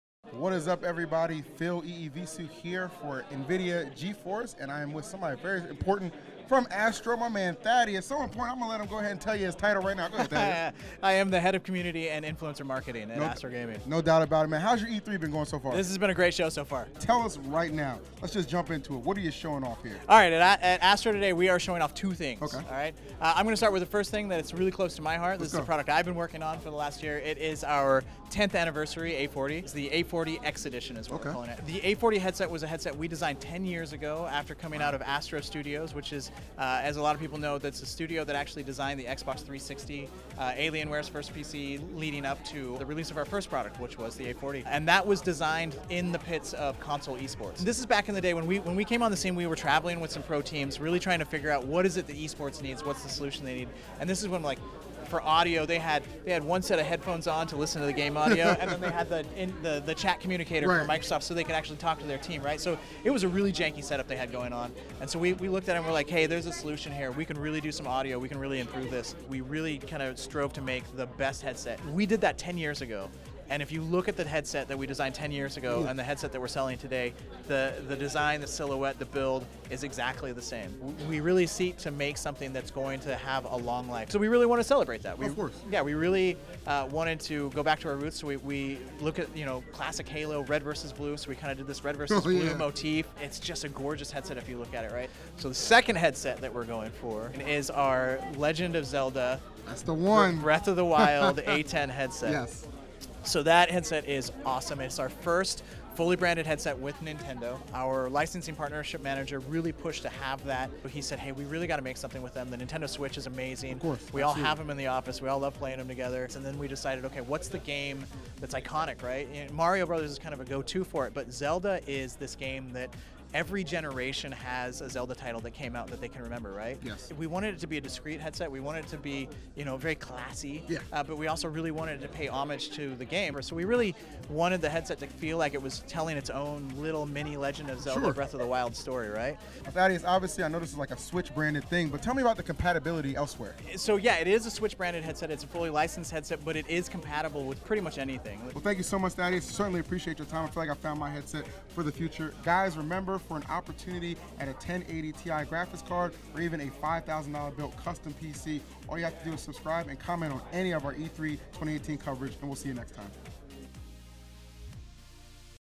Zelda: Breath of the Wild A10 Headset Coming From ASTRO!
We caught up with ASTRO to check out some of the awesome new headsets they have on the way. On top of their quality A40, they showed off their The Legend of Zelda: Breath of the Wild A10 headset. Although branded for the Switch, these bad boys will work great on your PC, too!

This video edited on a MSI GS63VR laptop with NVIDIA Max-Q design.